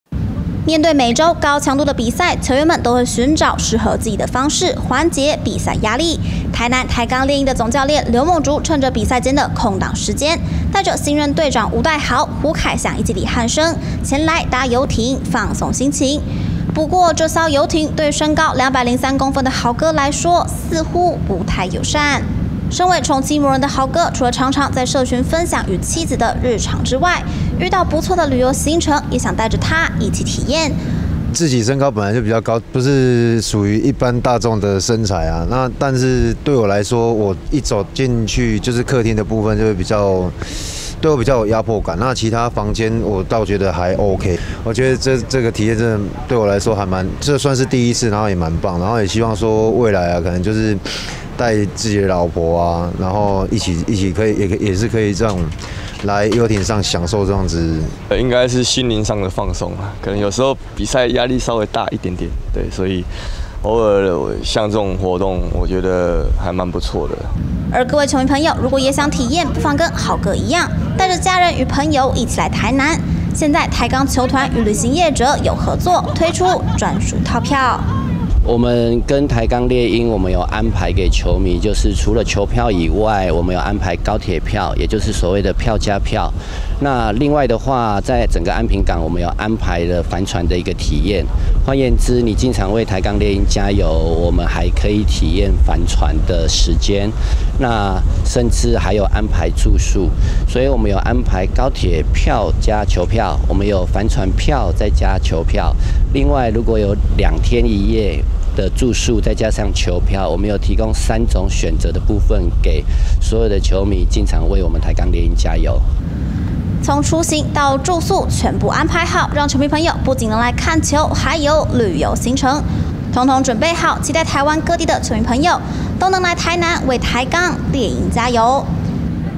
11/13 獵鷹眾將遊艇初體驗 寵妻魔人豪哥想帶老婆搭遊艇
職籃球員每週都要面對高壓比賽，因此需要一些休閒活動來種紓壓，比如打打電動，唱唱個等等，不過臺南台鋼獵鷹帶球員舒壓的方式不太一樣。

對他們帶球員去搭遊艇，體驗一下航海吹吹風放鬆一下，據說隊上很有名的寵妻魔人，也是獵鷹新任隊長吳岱豪，搭完之後也想帶上老婆一起體驗。